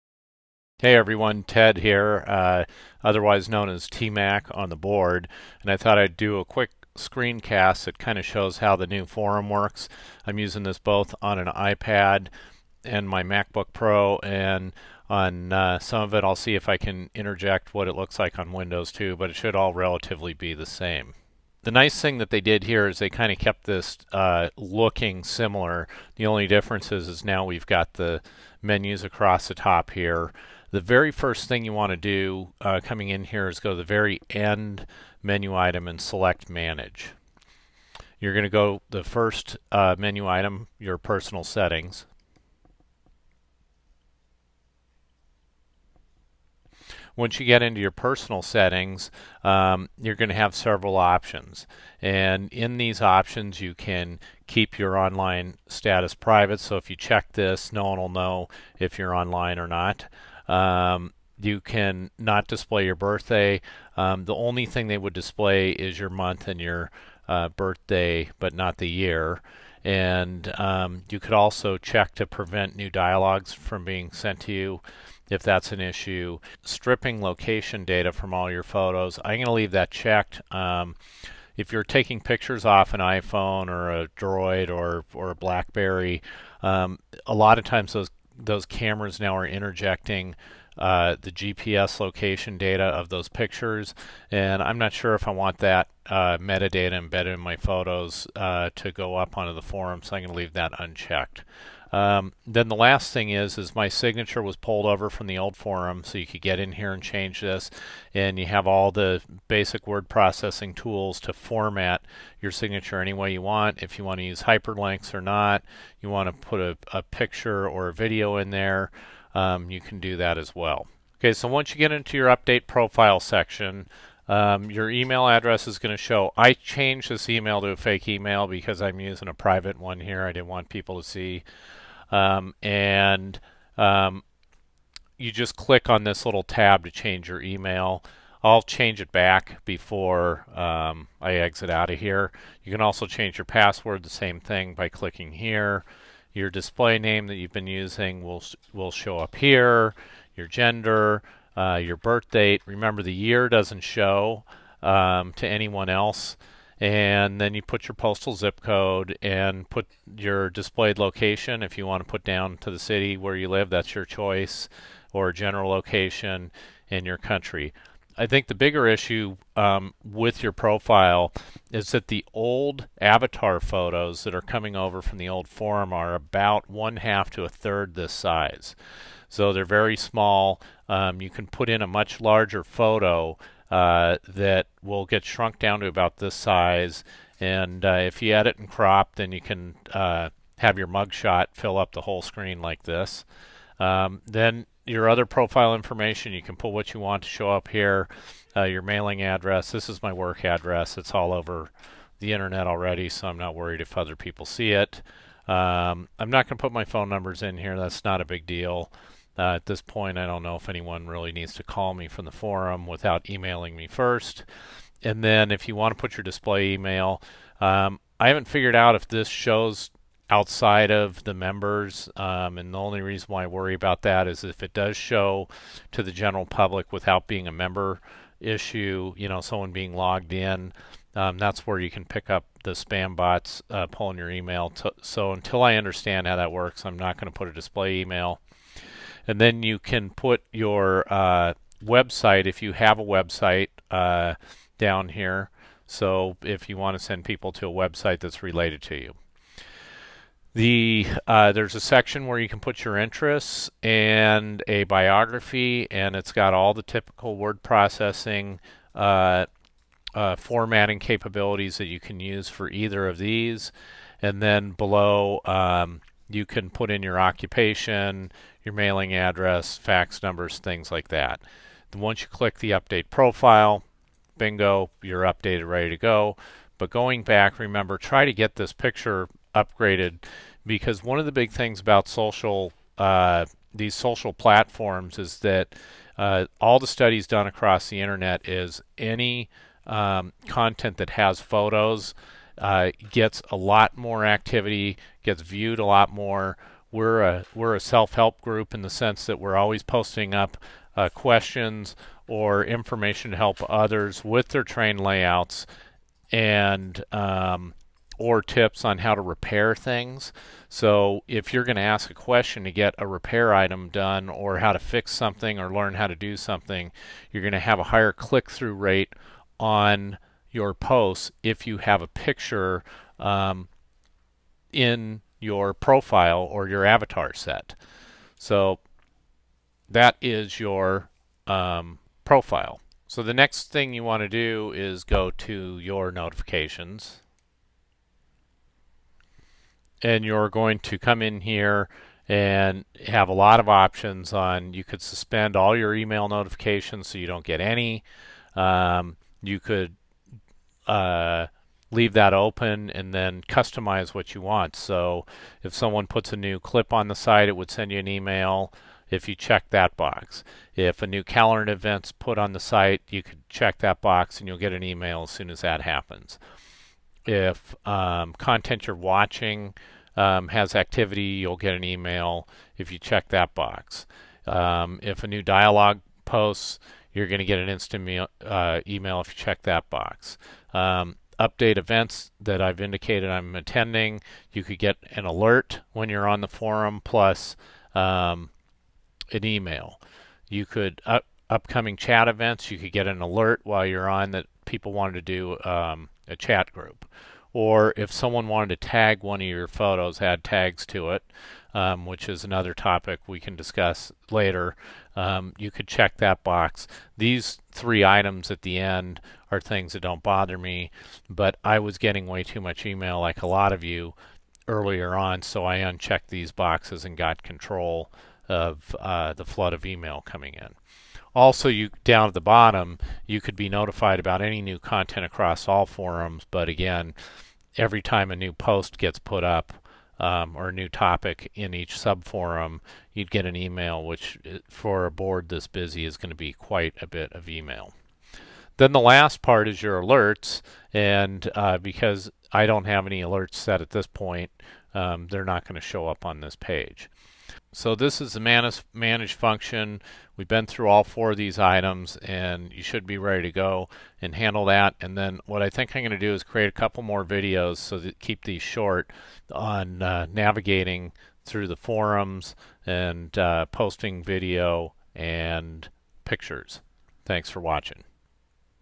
O Gauge Railroading Forum Tutorial Part 1
OGR upgraded their forum software.  I am creating a set of videos that will walk you through how to get going on the forum.  This video focuses on setting up your profile and notification setting.